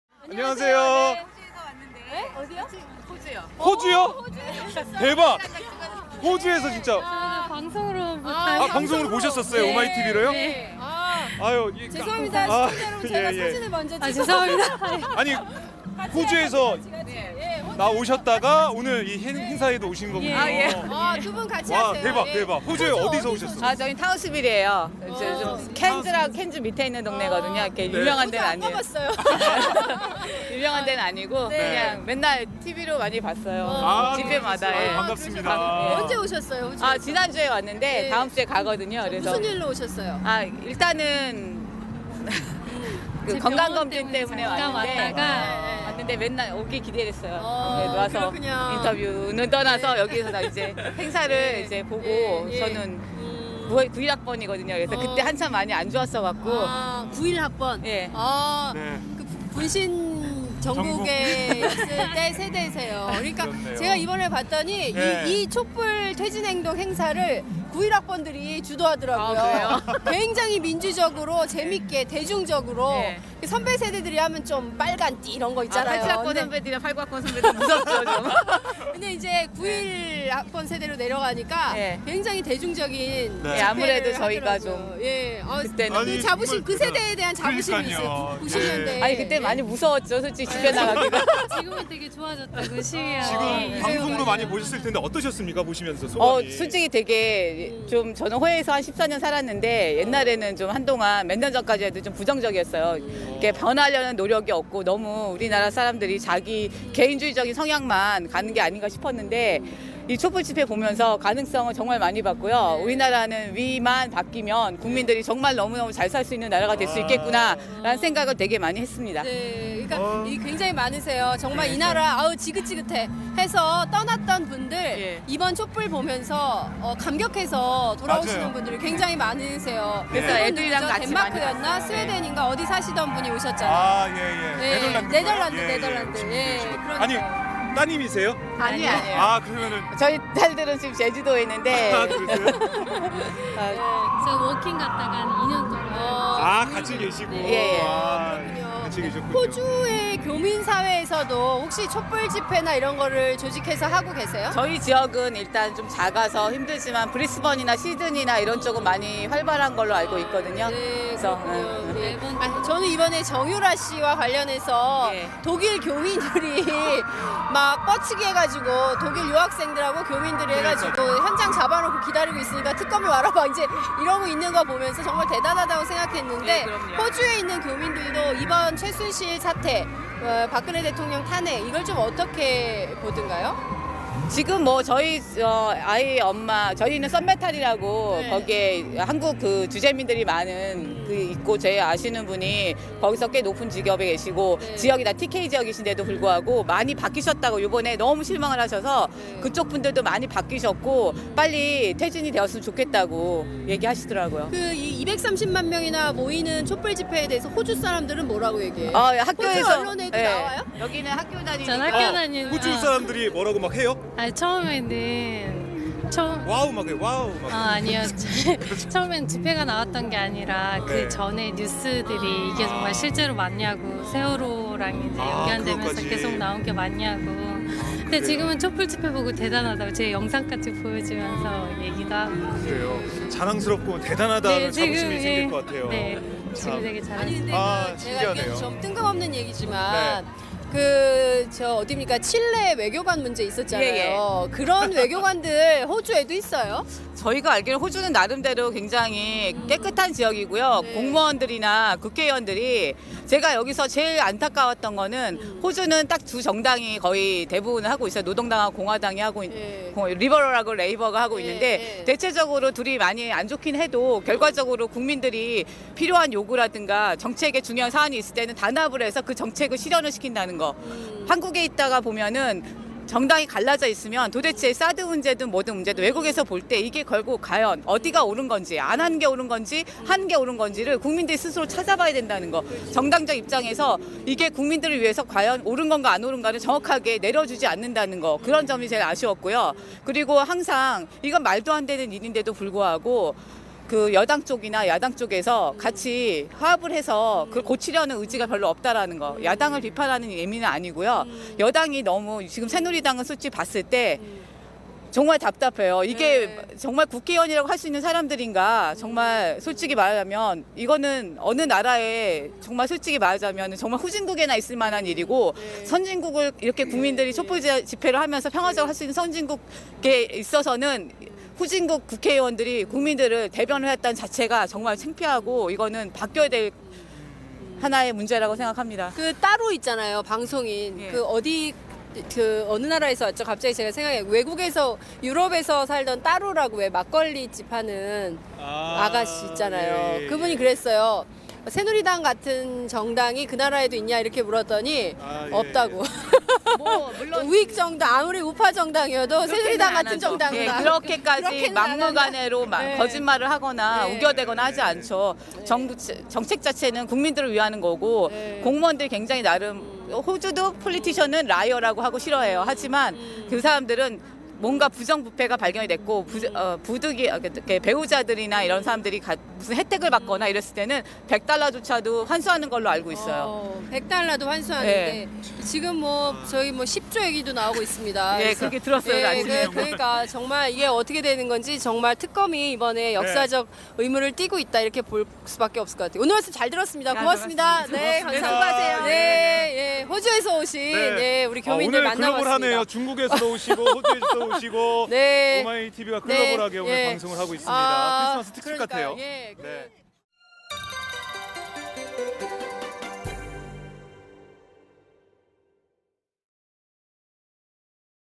[9차 범국민행동] 호주에서 온 시민 "새누리당 국회의원들은 후진국 수준"
24일 서울 종로구 광화문 광장에서 ‘박근혜 퇴진 9차 범국민행동’이 열렸다.

이 영상은 이날 공식행사 전 시민들과의 인터뷰를 담고 있다.

(취재 : 장윤선, 박정호 기자 / 영상 취재 : 윤수현, 조민웅 기자 / 영상 편집 : 이승열 기자)